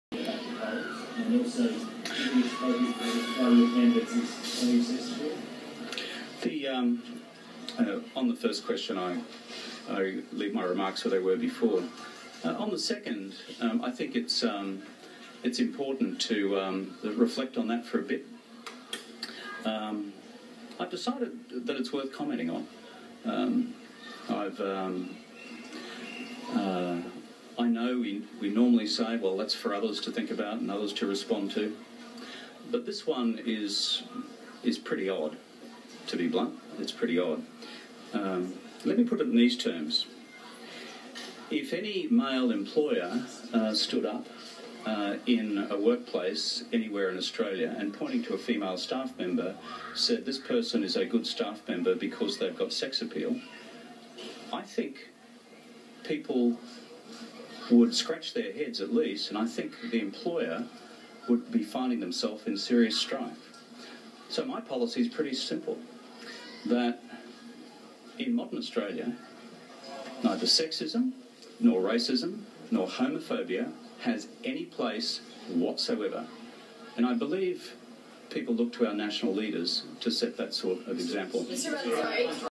Kevin Stalling Before Answering Against HQ Warning !
Kevin Rudd Disobeying ALP HQ Warnings about Commenting on Sex Appeal Comment. Notice the umm err before answering. Notice the shifty look over Shoulder. Was he worried the ALP Heavies might get him.